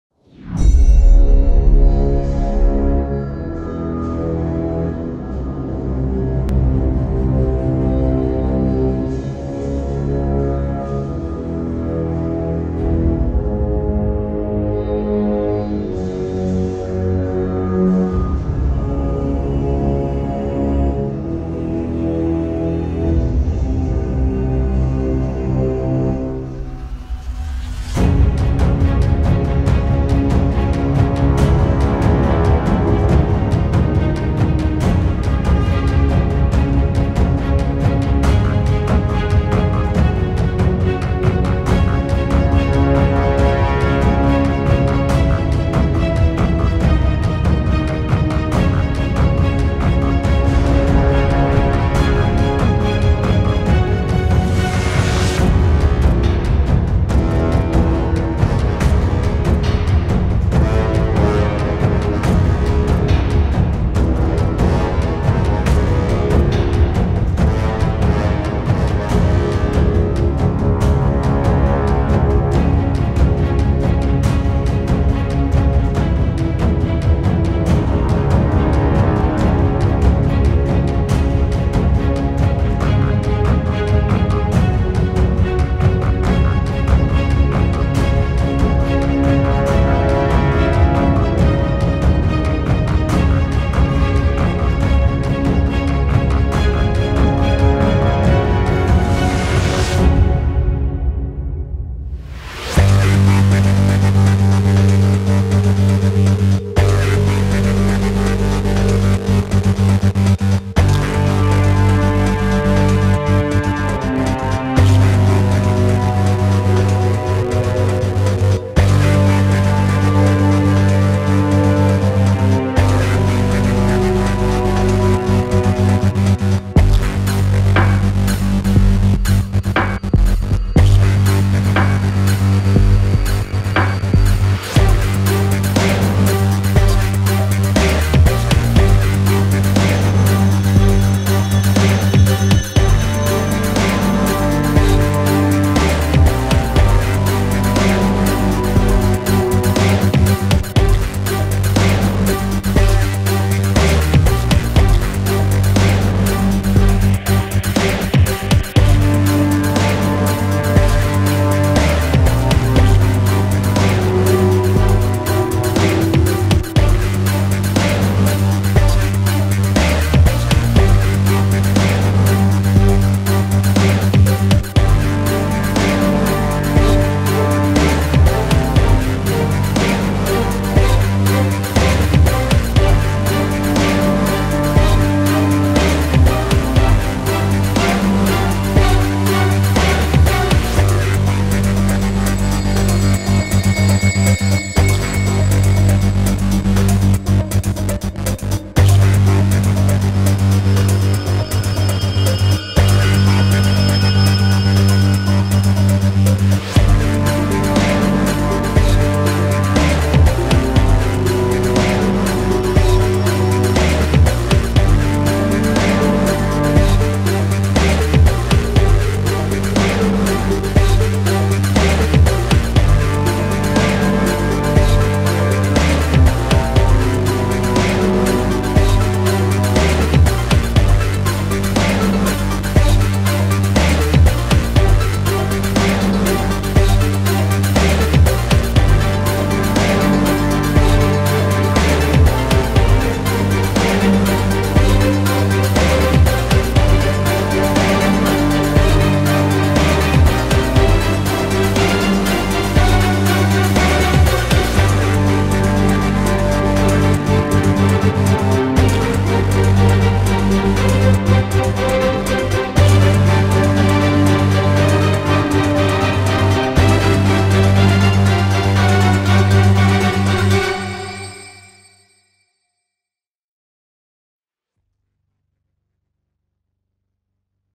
CyberPunk 2077 - Samouraï - Photoshop Speed Art
Design sur Photoshop d'un Samouraï ambiance CyberPunk 2077...

Dites moi ce que vous en pensez dans les commentaires :)

A bientôt